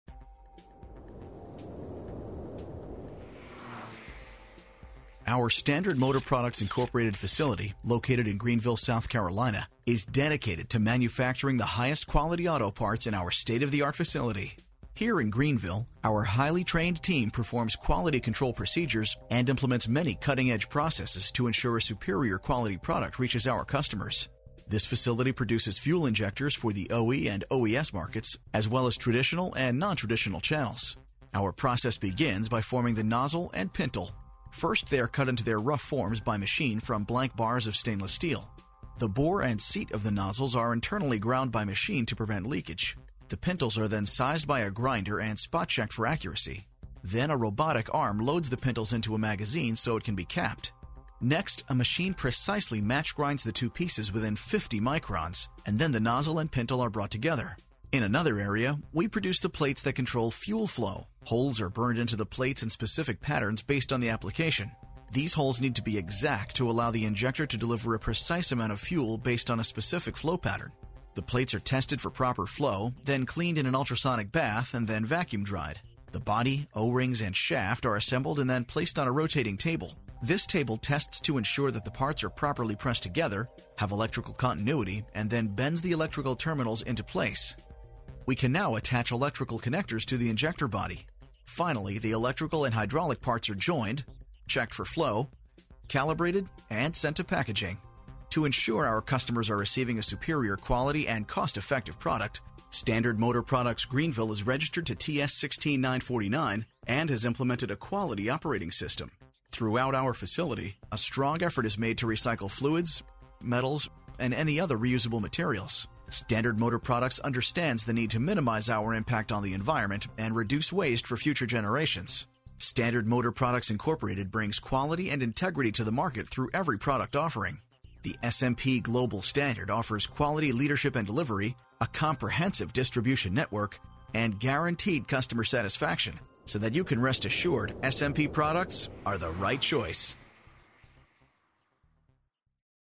SMP Fuel Injection Manufacturing, Greenville, SC, USA
The SMP business unit located in Greenville, SC maintains a state-of-the-art facility for the manufacture of fuel injectors, HEI ignition coils, ignition coil-on-plug units, camshaft/crankshaft sensors, and ABS sensors. These products are manufactured for distribution to the traditional aftermarket as well as the original equipment and original equipment service market channels, and industrial, marine and heavy-duty markets.
This large facility offers a complete engineering and testing operation along with highly vertical integrated manufacturing which includes injection molding, insert molding, machining, brazing, laser welding and a clean room. Our Greenville facility was the first SMP plant to attain ISO/TS 16949 quality standard.

Throughout our Greenville facility, a strong effort is made to recycle fluids, metals and all other reusable materials with a commitment to recycling all unused scrap parts so they may find a new life elsewhere. Standard Motor Products, Inc. understands the need to minimize our impact on the environment and reduce waste for future generations.